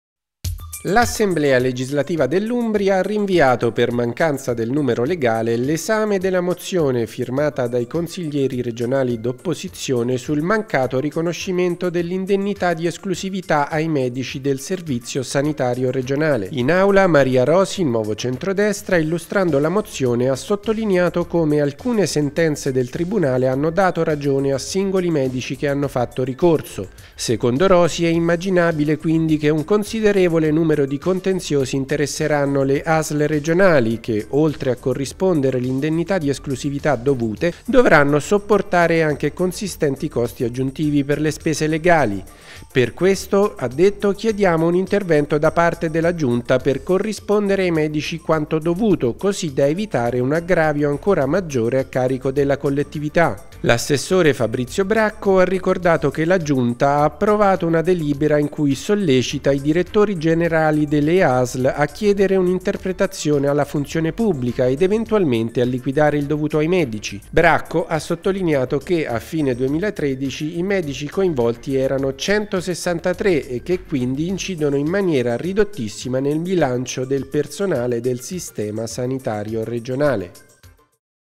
Indennità per i medici, mozione rinviata - seduta del 6 agosto 2014
L'Assemblea legislativa ha rinviato, per mancanza del numero legale, l'esame della mozione, firmata dai consiglieri regionali d'opposizione, sul mancato riconoscimento dell'indennità di esclusività ai medici del servizio sanitario regionale.